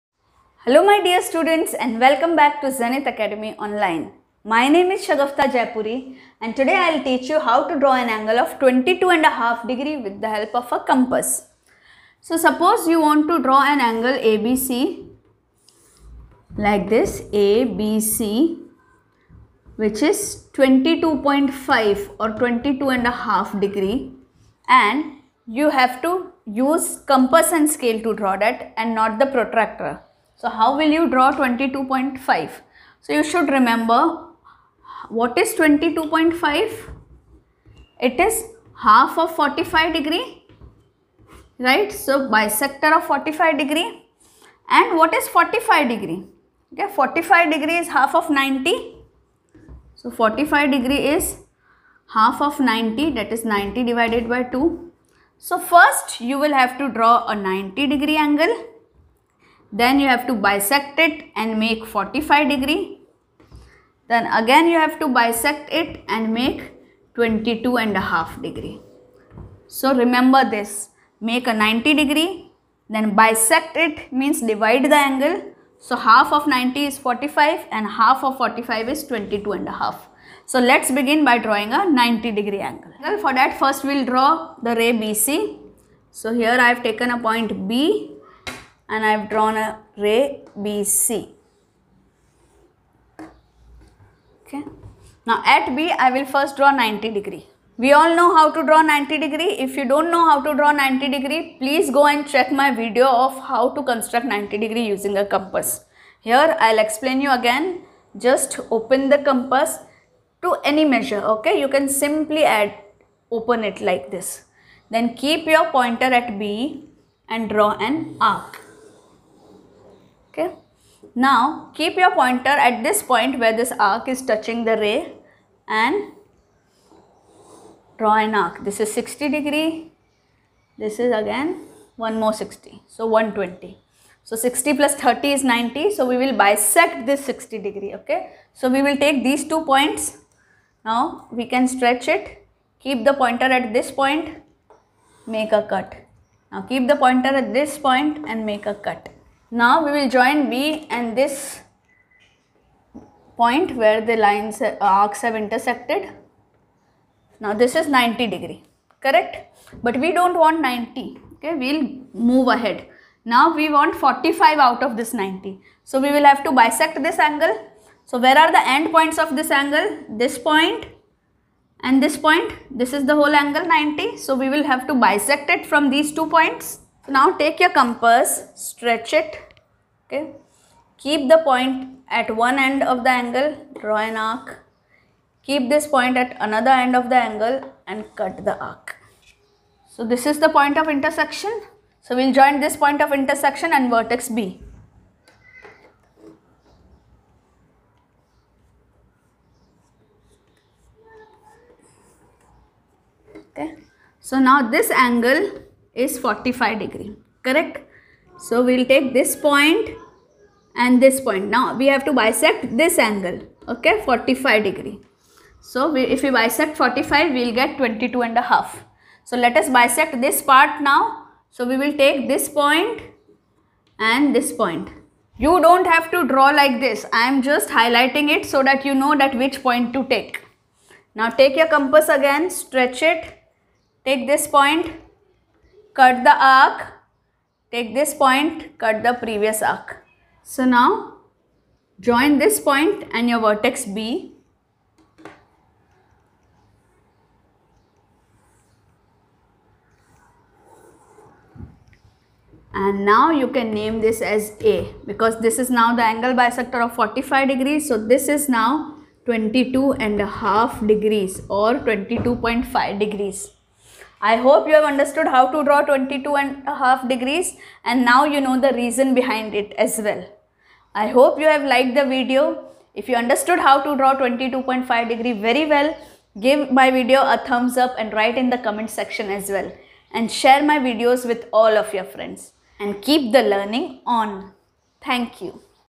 Construct an angle of 22.5° using Compass
Construct an angle of 22° using Compass and scale very easily with full concept and reasoning